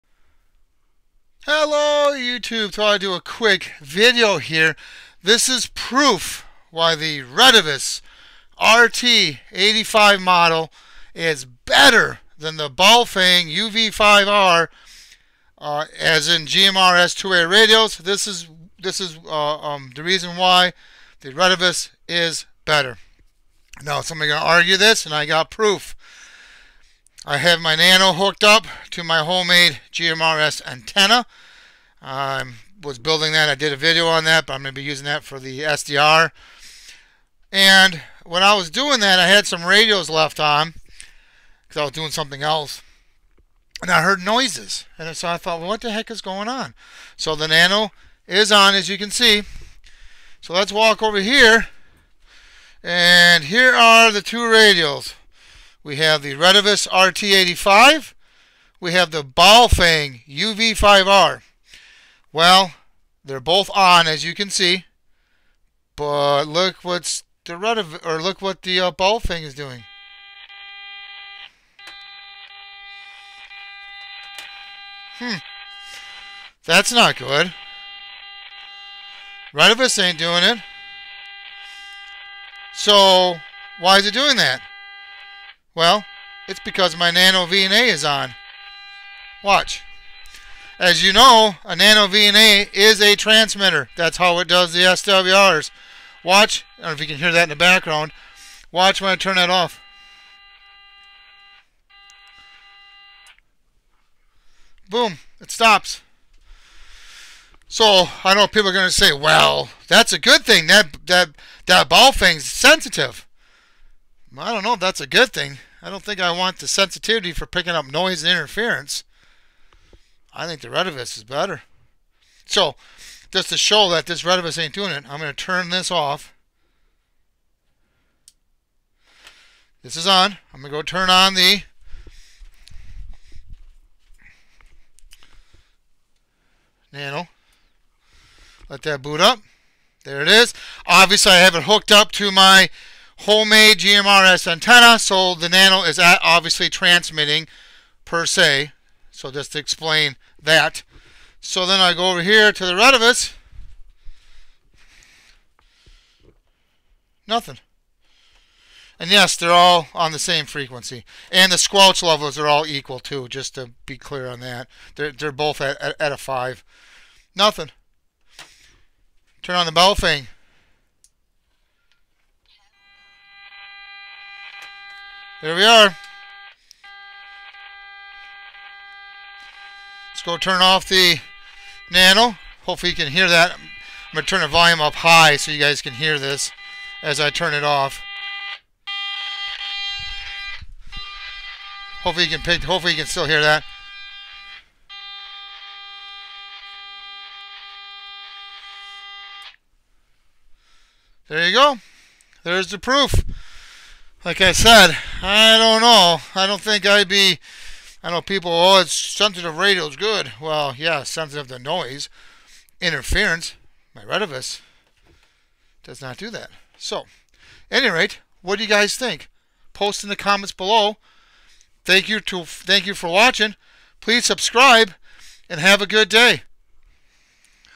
🔺 GMRS two-way radio the Retevis RT85 is BETTER than the Baofeng uv-5r this is why!!!🔺
in this video, I show you PROOF why the Retevis RT85 GMRS two-way radio is BETTER than the Baofeng UV-5R!

NOTE: it didn't matter what channel the Baofeng UV-5R was on!! it still did the noise!!